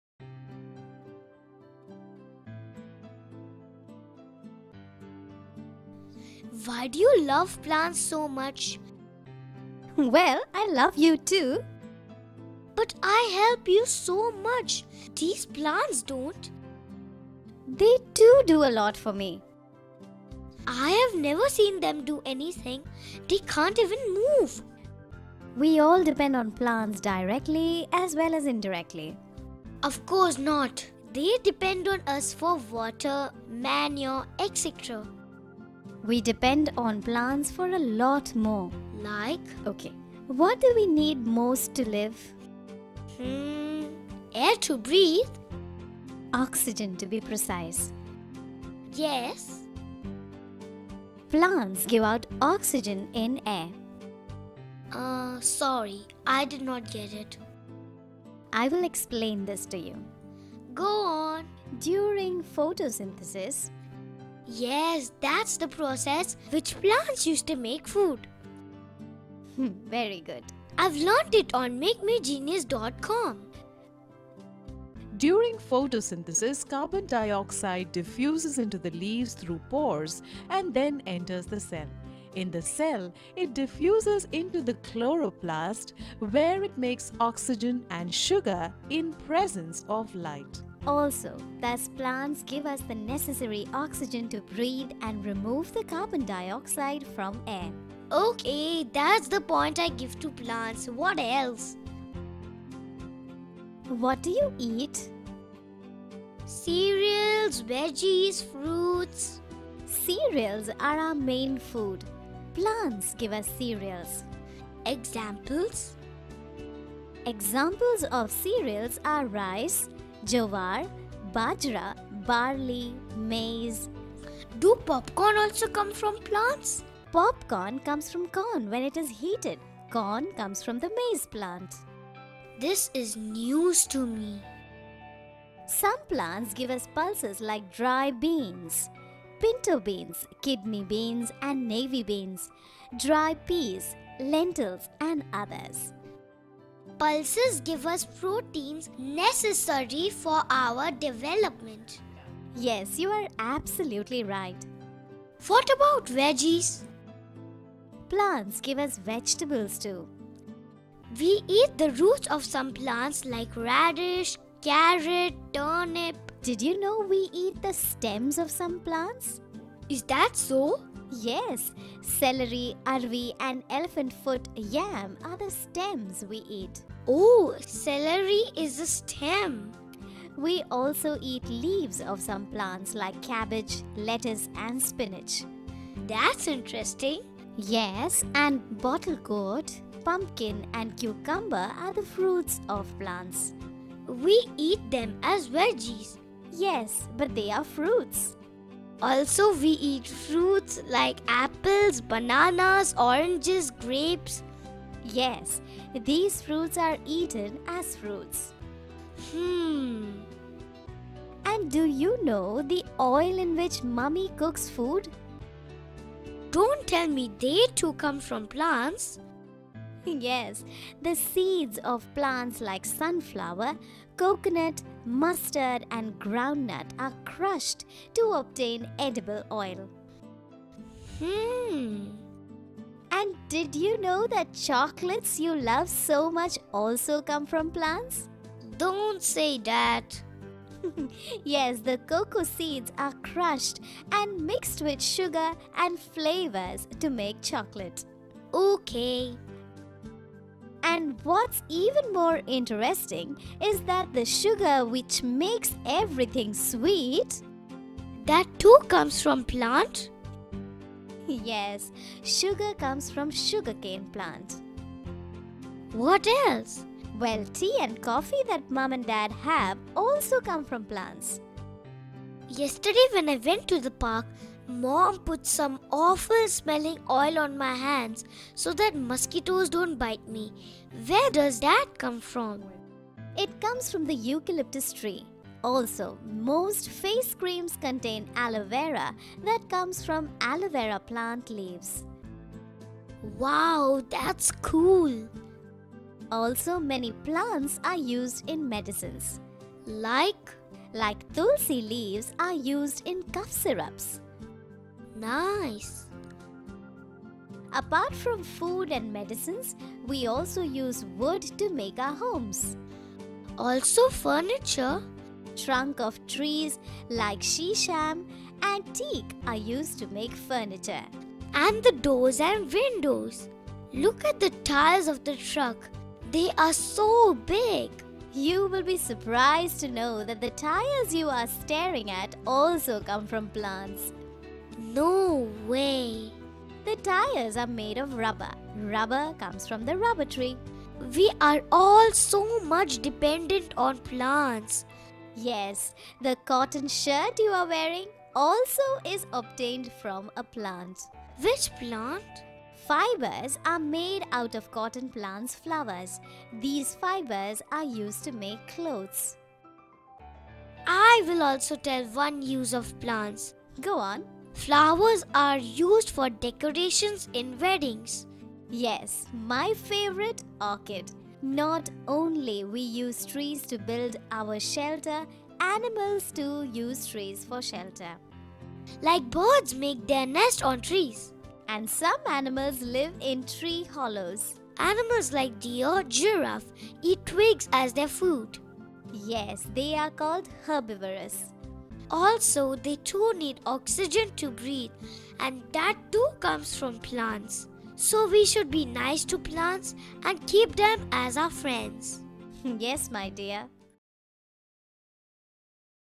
Uses of Plants for Kids
Kids can learn about various uses of plants in this education video.